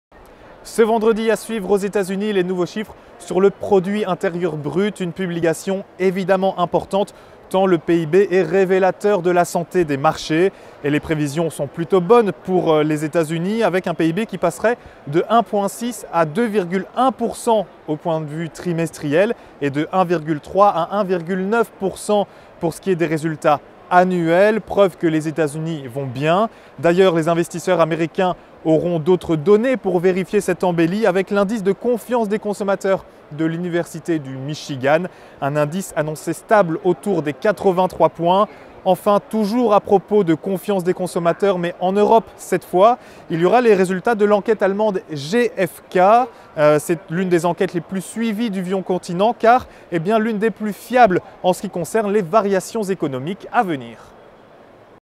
A suivre : nouveaux chiffres du PIB aux Etats-Unis
26/10/12. De nouveaux résultats sur le Produit Intérieur Brut seront dévoilés, vendredi, aux Etats-Unis. Une hausse, tant trimestrielle qu'annuelle, est prévue. En Europe, on suivra le bilan d'une étude allemande sur la confiance des consommateurs.